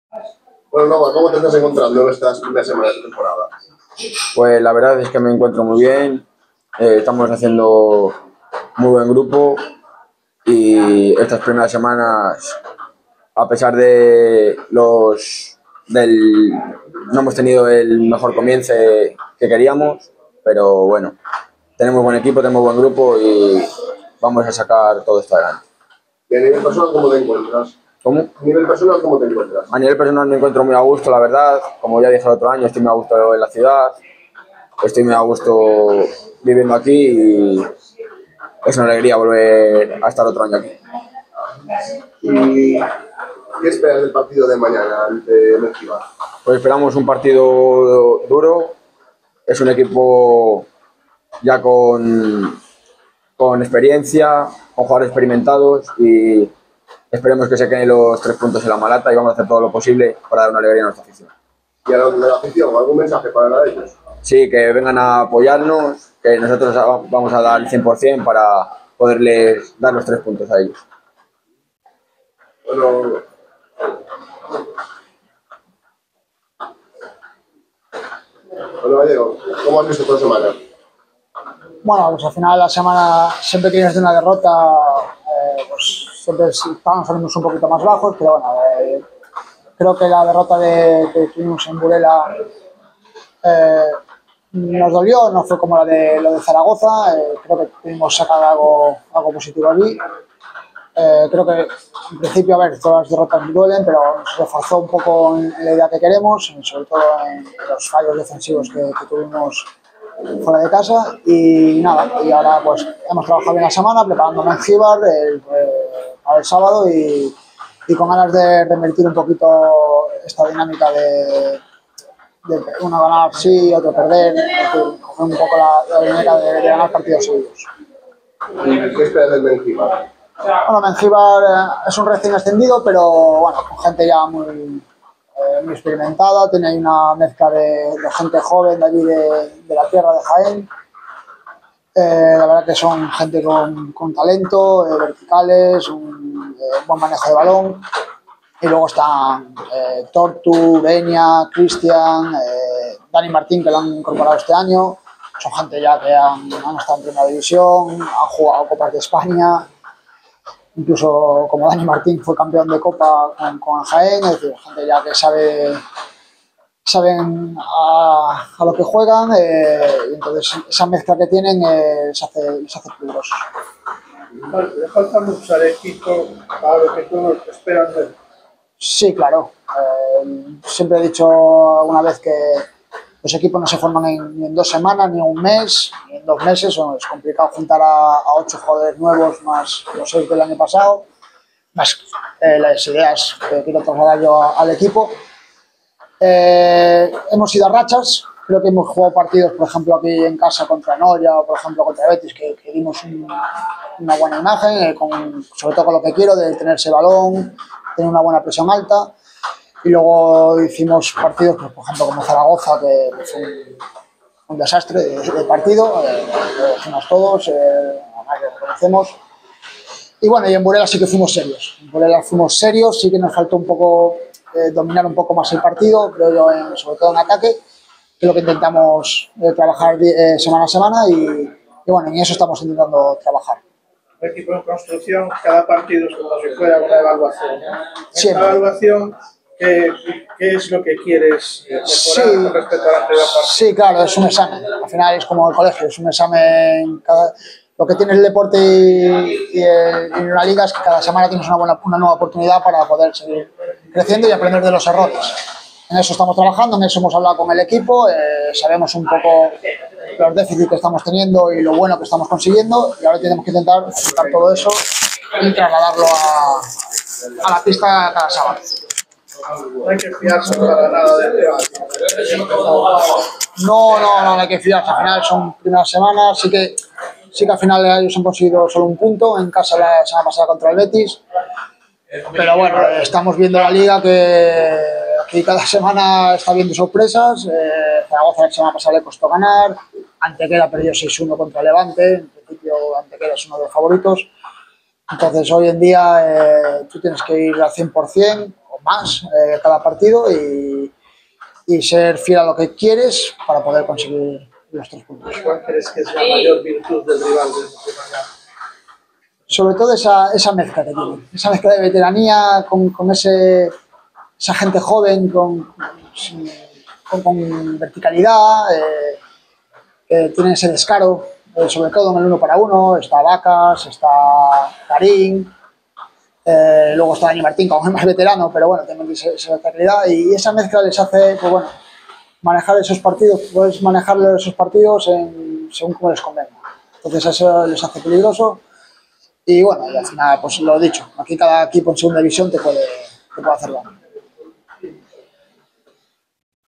Rueda de prensa de Novoa y Gallego en Restaurante de Manuel
El jugador de O Parrulo Ferrol, Novoa y el entrenador Gallego hablan en rueda de prensa antes de jugar ante Oleoinnova Mengíbar, en la 4ª jornada de la Segunda División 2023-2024, en un evento celebrado el viernes 13 de octubre de 2023, en Restaurante de Manuel, establecimiento patrocinador de O Parrulo Ferrol y su Escuela de Fútbol Sala.